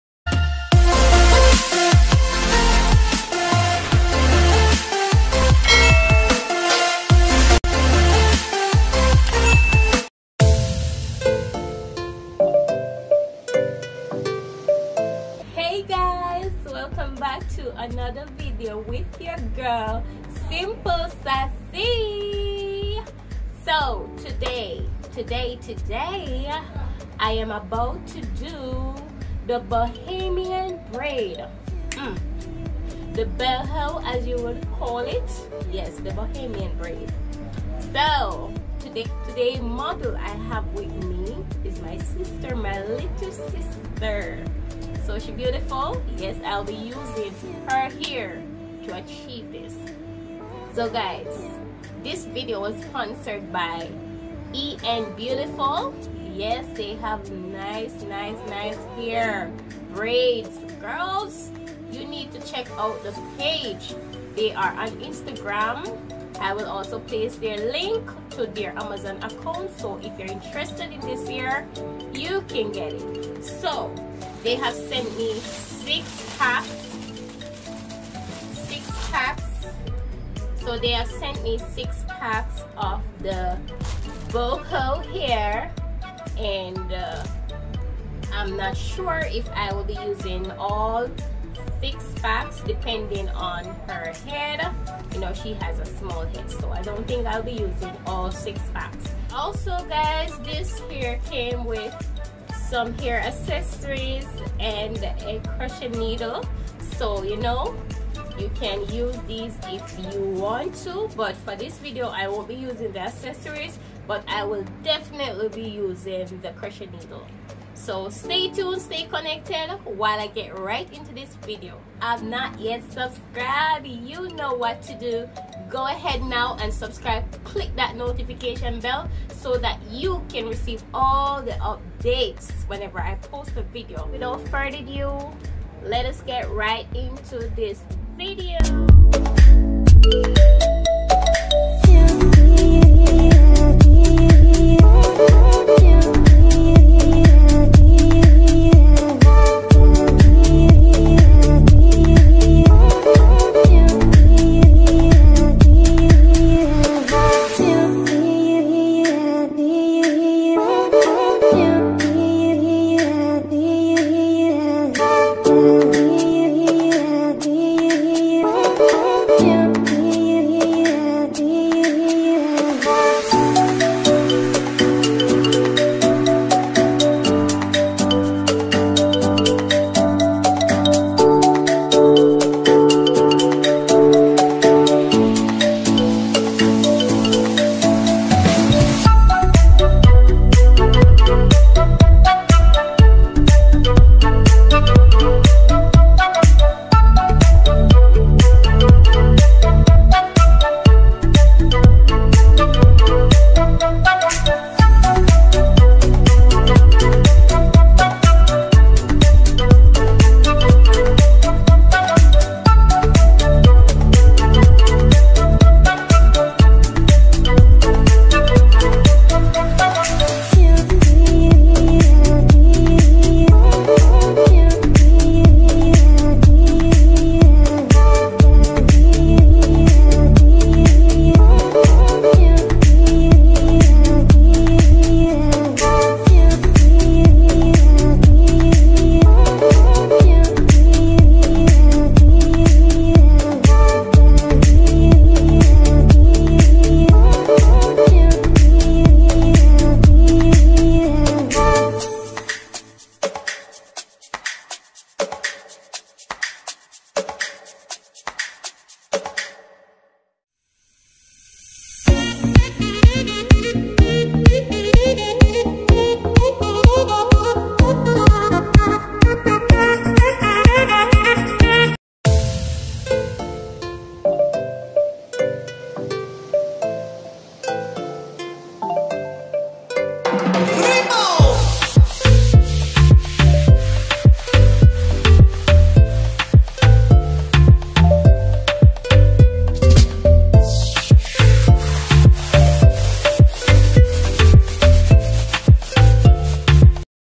How To Goddess Boho CROCHET Boxbraidstutorial bohobraids Ft. Enbeautiful
Hey guys!

HAIR DESCRIPTION:

14 Inch 8 Packs Goddess Boho Box Braids Gypsy Bohemian Braids With Wavy Curly Ends Pre-looped Synthetic Crochet Hair For Black Women(14inch, 8packs,  COLOR -1b

Product Used:

📌Crochet needle
📌 Boho Braids:
* 8 pks- 12 strands per pk

purchase hair here:

Purchase Butterfly Locs here:

@Lisa Wallace